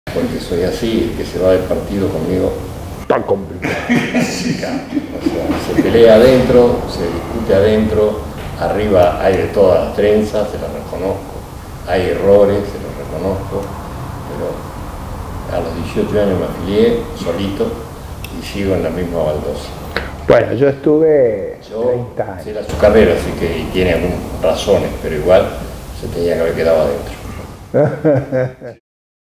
EL INDIVIDUO VS. EL ESTADO - LIBERTAD DE EXPRESIÓN
En la visita de Lopez Murphy a Tandil, el Diputado Nacional propone una reunión con el
Intendente M.A.Lunghi., este no aparece a la hora fijada por el secretario, lo llaman y se justifica que no le avisaron de la entrevista. Al pasar a la oficina del Intendente lo saludo cordialmente, para luego desempeñar mi trabajo de camarógrafo del encuentro. Luego de hacer esta toma donde el Intendente dice que es complicado con las personas que se van del partido, no dejándolo hablar a L.M., señalándome a mi, se dirige a los demás diciendo...quien invito a esta persona, tengo problemas con este señor, dirigiéndose a mi. Tuve que dejar mi labor como camarógrafo, en una reunión que no tenía nada de secreto de estado, para no llevarla a un plano de conflictos personales. Por supuesto antes de irme aclaré que no era el lugar para entablar esta discusión y que estaba dispuesto a debatirlo en otro ámbito como puede ser un canal de TV. Cosa que nunca aceptó el Intendente M.A.L.
¿El pueblo quiere saber la verdad? o la verdad molesta?